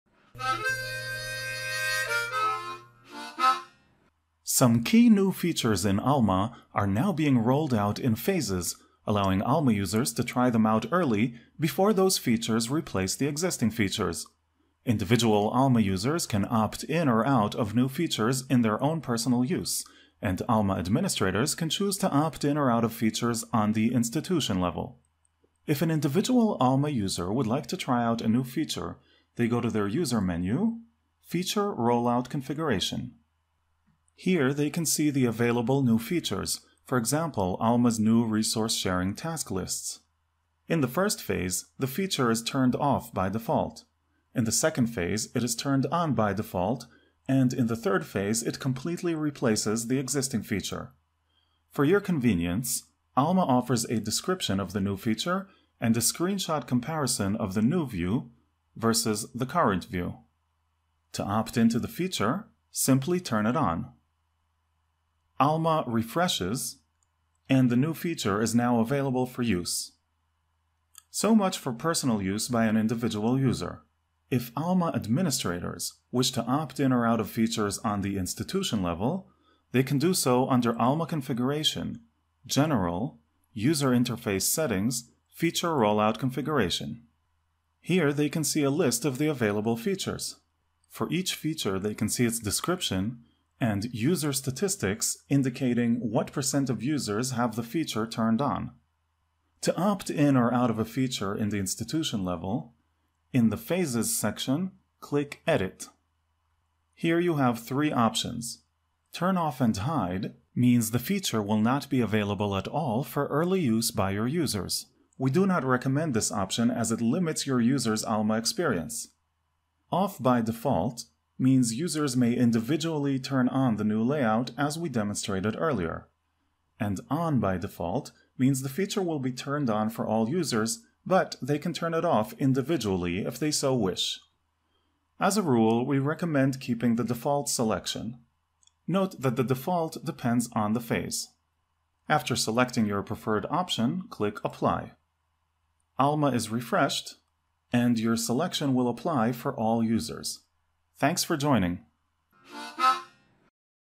Alma August 2022: Opting In or Out of a New Feature
Some key new features in Alma are now being rolled out in phases, allowing Alma users to try them out early, before they replace the existing features. Individual Alma users can opt in or out of new features in their personal use; and Alma administrators can opt in or out of features on the institution level.